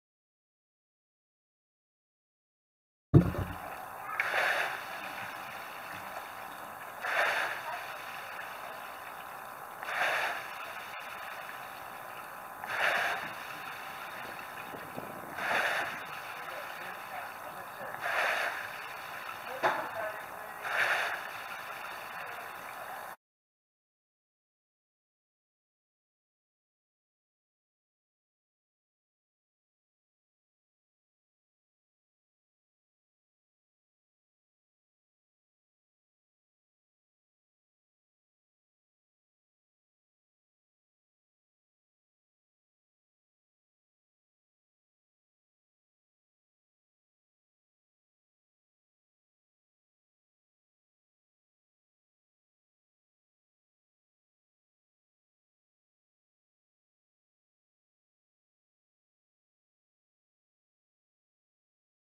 Egg Ergs
Rowers having fun with ergos at Easter during a time of coronavirus.